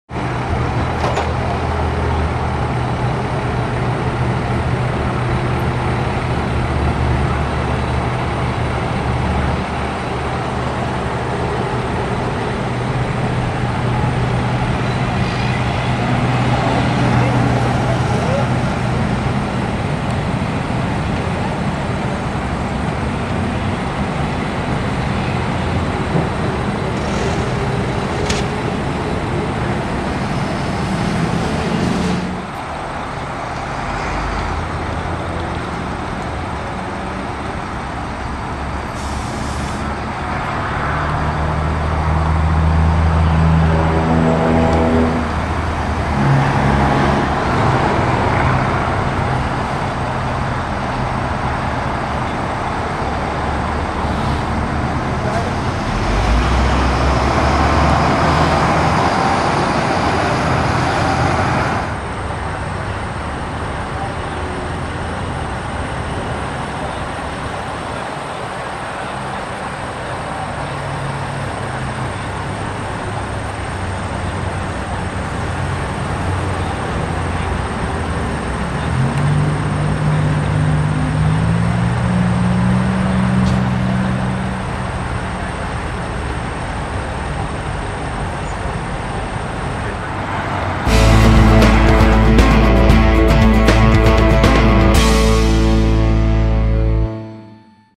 Transport Truck for Refrigerators, Dryers, Washing Machines and other Appliances Catches Fire
Truck fire at the exit ramp of Route 53 to to Rand Road Route 12.

Full story at The Cardinal ...